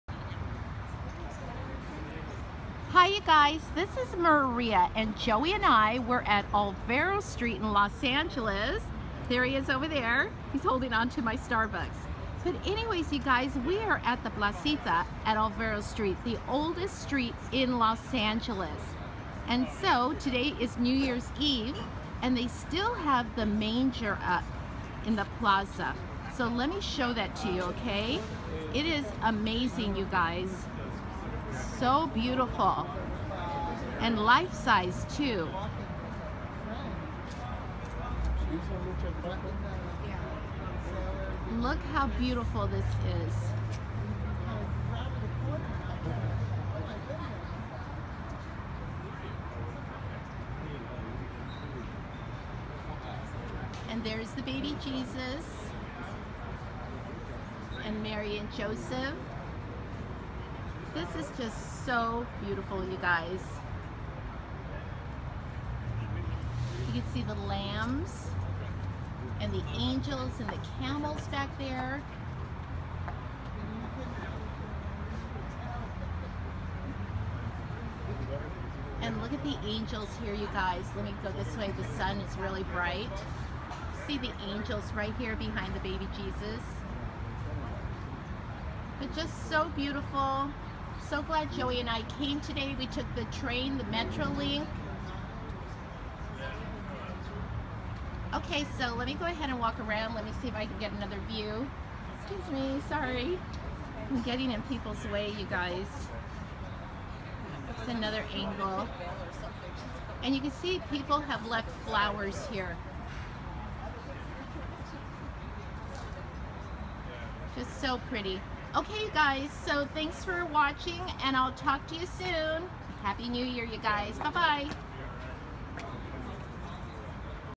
Lifesize Nativity at La Placita, Olvera Street!
New Year's Eve, my husband, Joey and I took the Metrolink train to Los Angeles and spent the day at Olvera Street, a historical landmark that is the oldest street in Los Angeles. Across the street from the Mexican marketplace is La Placita. In the center of the plaza is a large gazebo where we found a life-size Nativity scene. It was so beautiful, so I wanted to share it with you! And I hope you all have a wonderful, healthy, happy, prosperous, and creative 2018!!! Learn more about children's author, Maria Cisneros Toth